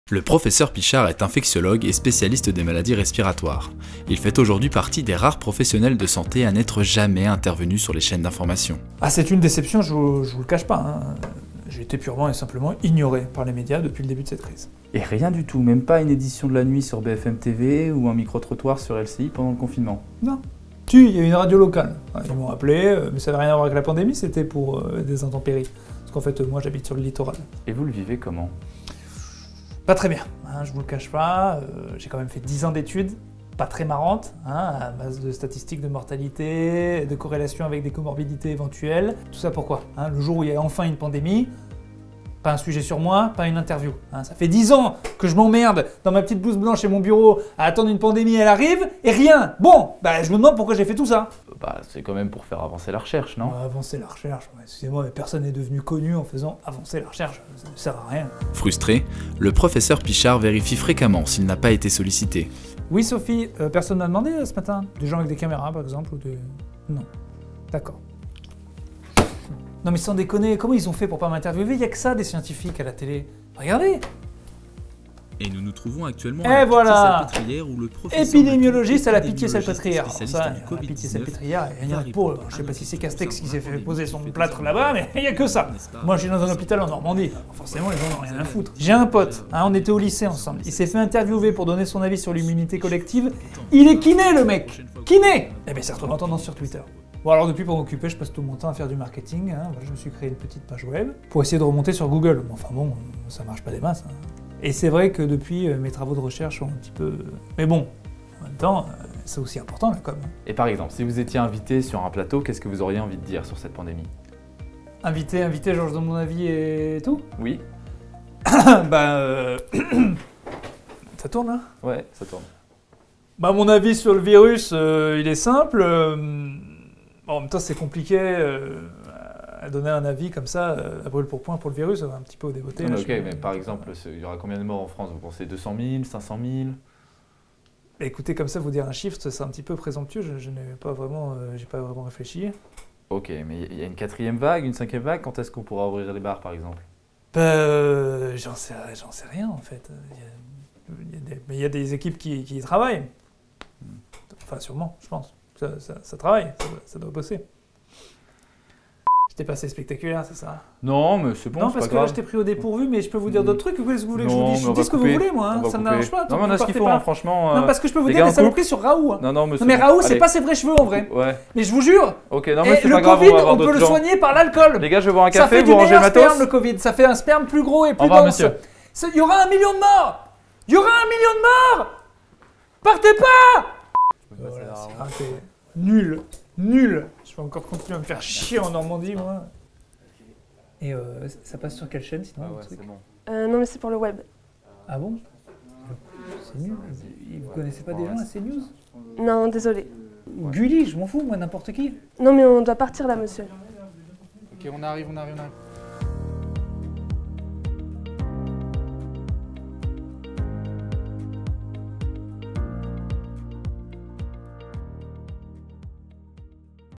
L'infectiologue jamais passé à la télé
Il n'en reste plus qu'un, et on l'a trouvé : le seul infectiologue jamais passé à la télé.